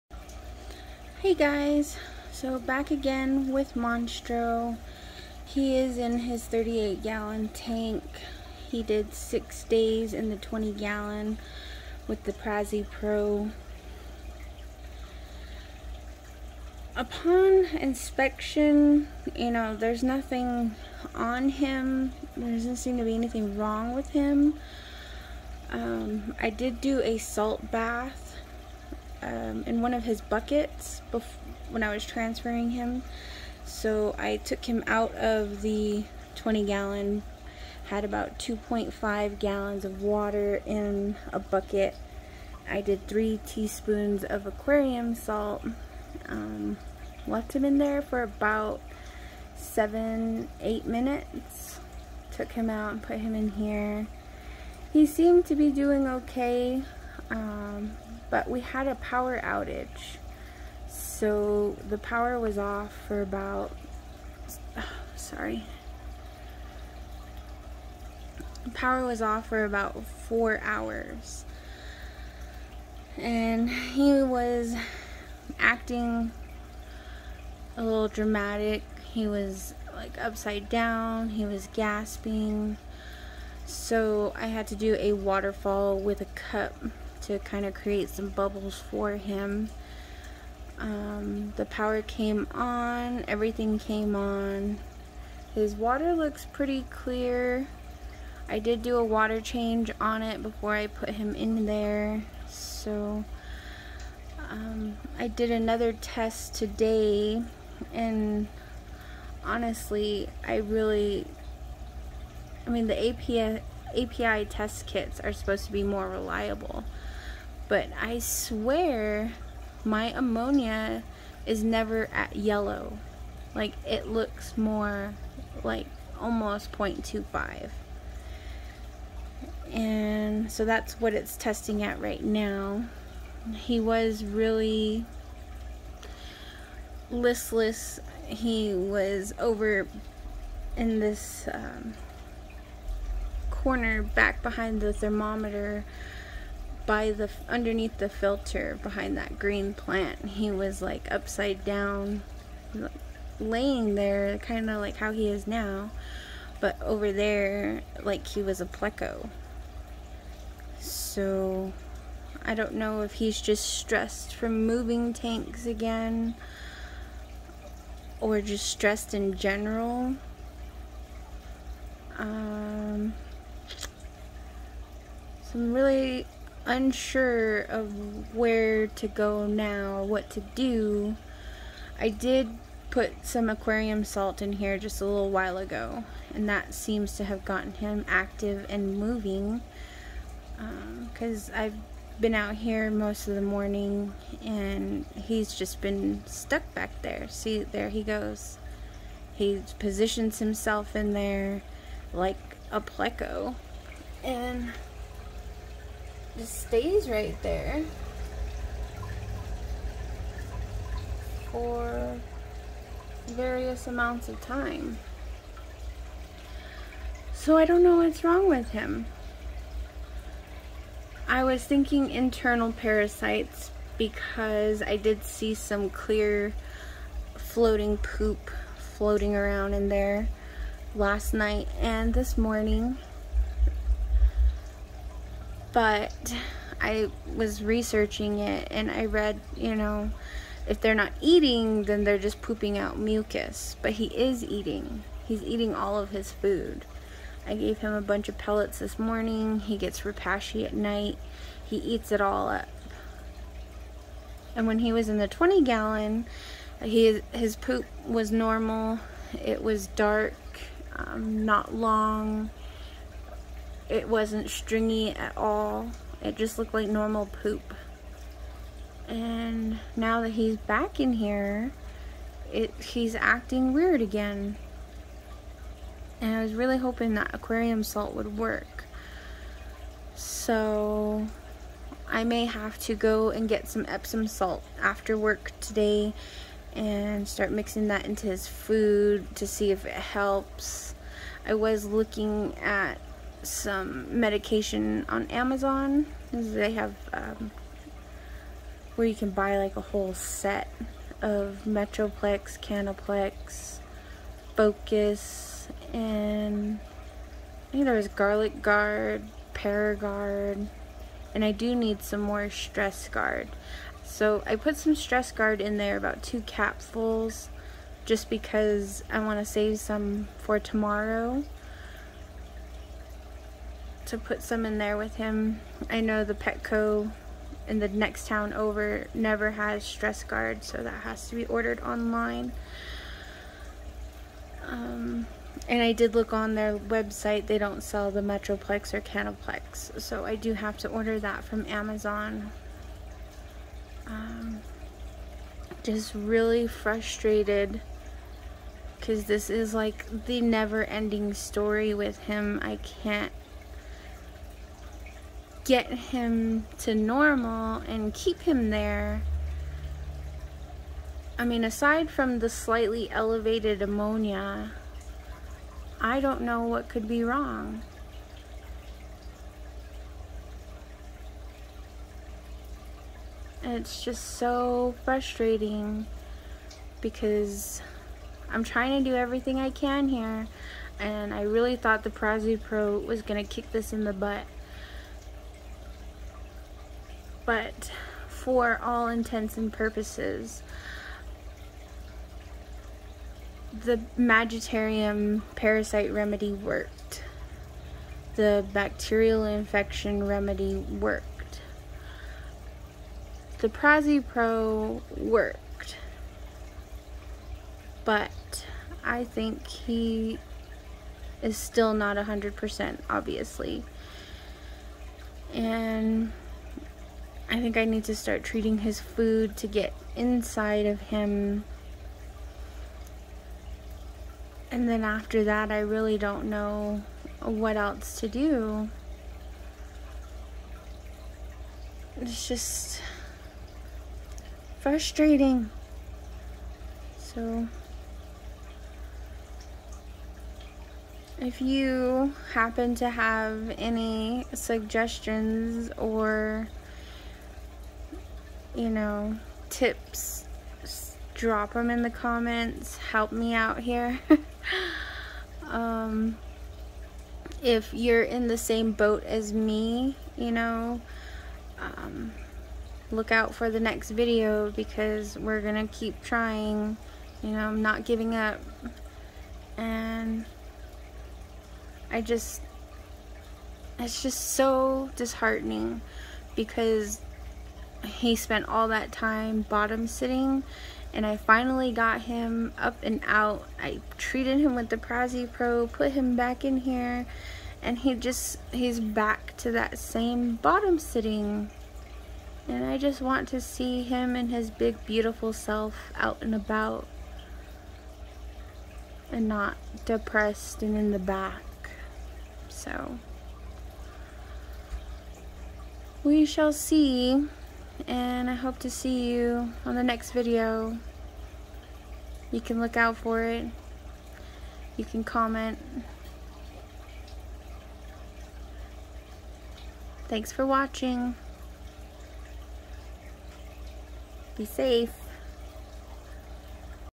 PraziPro Update 3
Monstro is out of medicated water and back in his 38 gal tank after a PraziPro treatment. Doesn't look like we're out of the woods yet though...